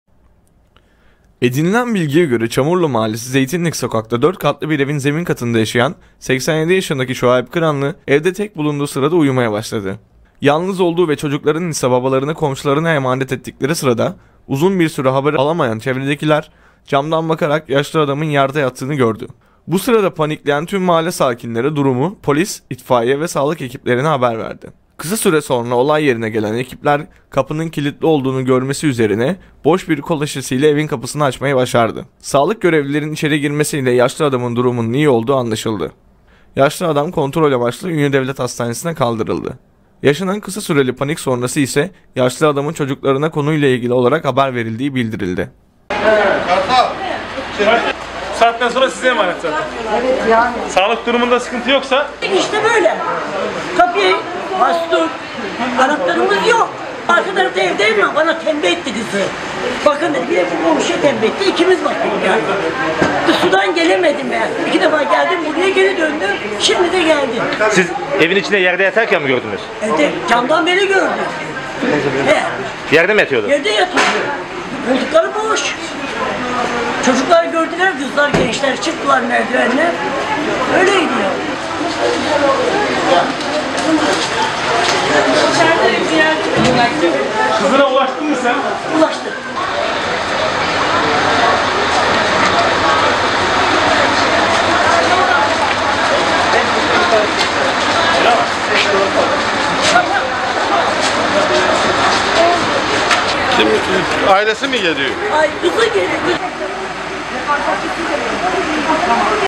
O Uyudu Mahalleli Ayağa Kalktı
Ünye'de evinin ortasında uyuya kalan yaşlı adam, mahalleliyi ayağa kaldırdı.
Edinilen bilgiye göre, Çamurlu Mahallesi Zeytinlik Sokak'ta 4 katlı bir evin zemin katında yaşayan 87 yaşındaki Şuayip Kıranlı, evde tek bulunduğu sırada uyumaya başladı. Yalnız olduğu ve çocuklarının ise babalarını komşularına emanet ettikleri sırada uzun bir süre haber alamayan çevredekiler camdan bakarak yaşlı adamın yerde yattığını gördü. Bu sırada panikleyen tüm mahalle sakinleri durumu polis, itfaiye ve sağlık ekiplerine haber verdi.
Kısa sürede olay yerine gelen ekipler kapının kilitli olduğunu görmesi üzerine boş bir kola şişesiyle evin kapısını açmayı başardı. Sağlık görevlilerinin içeri girmesiyle yaşlı adamın durumunun iyi olduğu anlaşıldı. Yaşlı adam kontrol amaçlı Ünye Devlet Hastanesi'ne kaldırıldı.
Yaşanan kısa süreli panik sonrası ise yaşlı adamın çocuklarına konuyla ilgili olarak haber verildiği bildirildi.